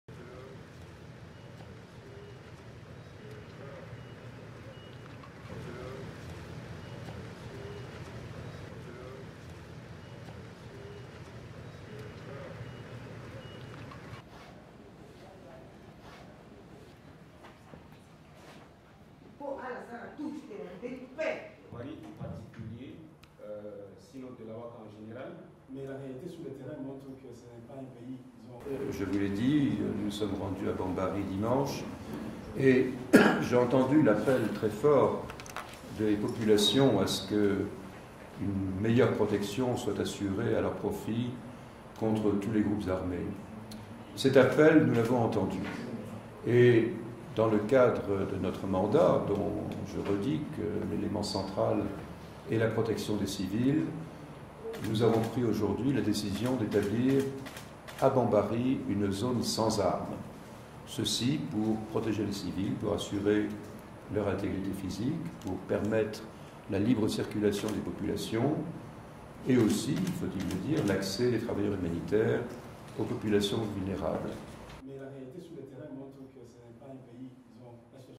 USG Herve Ladsous visit Bambari/Press Conference
Bangui, September 8, 2015 - The Under Secretary General of the United Nations in charge of Peacekeeping Operations, Herve Ladsous, has announced the establishment of an “weapons-free zone” in Bambari effective today, 8 September , 2015, from 17:00 local time.

The United Nations Multidimensional Integrated Stabilization Mission in the Central African Republic (MINUSCA) “took the decision to establish a weapons-free zone in Bambari to ensure the free movement and physical integrity of the civilian population, as well as access by humanitarian workers to vulnerable populations” he said.

This decision follows a call on Mr. Ladsous by the population of Bambari, on 6 September, for further protection against armed groups, without exception, who operate in the region.

“MINUSCA welcomes the commitment of the armed groups to respect this measure as a concrete way to improve the security of the population of Bambari, create conducive conditions for the return of displaced persons, and allow the start of voter registration in Bambari due to start on 14 September ”, said Mr. Ladsous. MINUSCA will oversee the implementation of this measure.

On 8 September, the Under Secretary General of the United Nations in charge of Peacekeeping Operations, ends his visit to the Central African Republic. The objective was to reiterate the support of the United Nations on the eve of the general elections